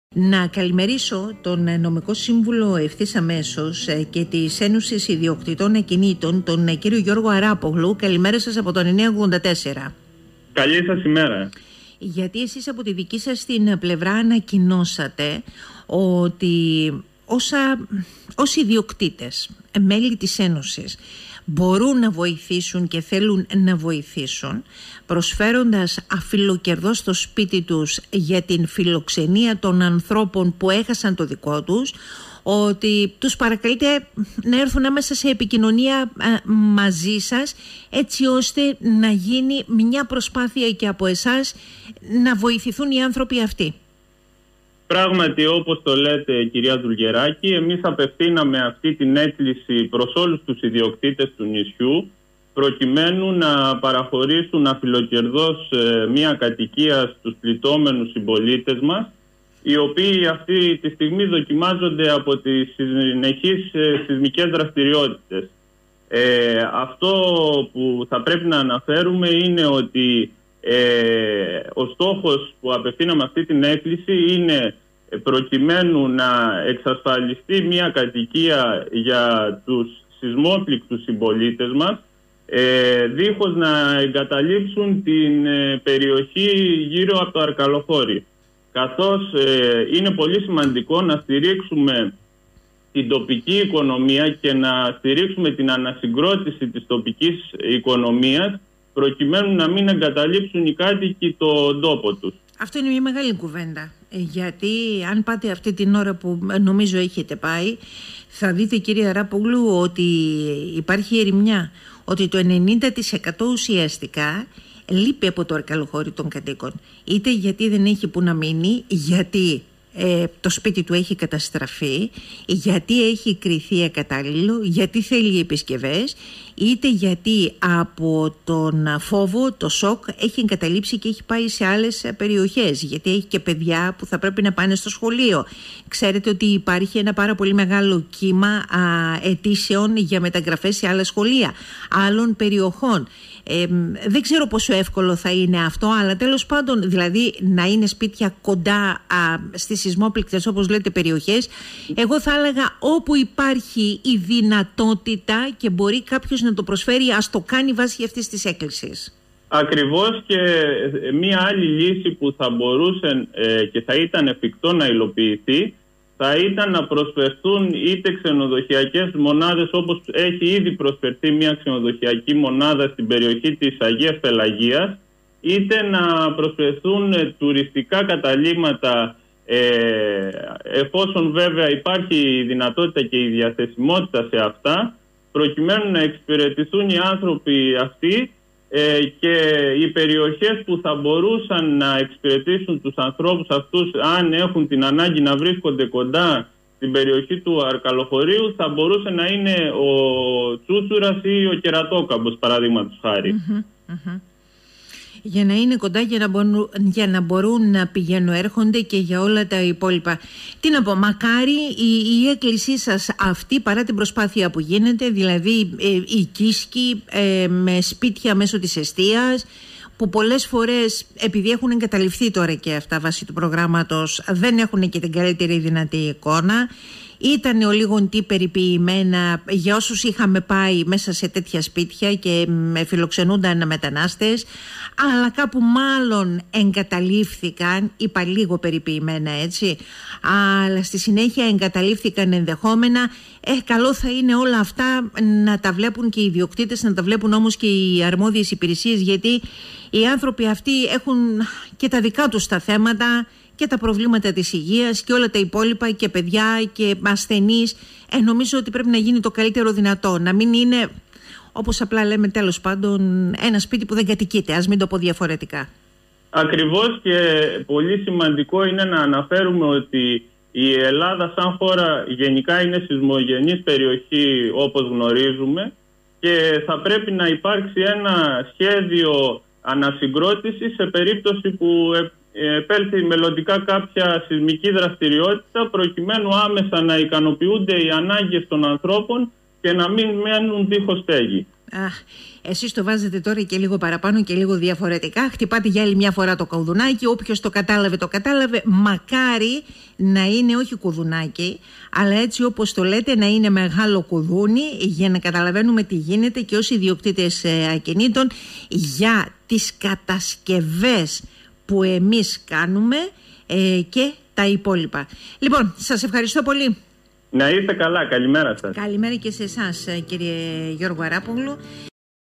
Γ. Αράπογλου: Ζητούμενο μας η στήριξη των σεισμόπληκτων
Η Ένωση ιδιοκτητών ακινήτων ζητά από τους ιδιοκτήτες να φιλοξενήσουν στα σπίτια τους αφιλοκερδώς πληγέντες
Όπως ο Νομικός Σύμβουλος της Ένωσης Γιώργος Αράπογλου αναφέρει στον 98.4: «Οι ανάγκες που έχουν οι σεισμόπληκτοι συμπολίτες μας πρέπει άμεσα να ικανοποιηθούν προκειμένου να έχουν τη δυνατότητα να τις εξυπηρετούν κάτω από μία στέγη. Προτιμώνται τα σπίτια να βρίσκονται στις σεισμόπληκτες περιοχές ή κοντά σ’ αυτές για να στηριχθεί η προσπάθεια ανασυγκρότησης της τοπικής οικονομίας, χωρίς να υπάρξει εγκατάλειψη της περιοχής από τους κατοίκους της».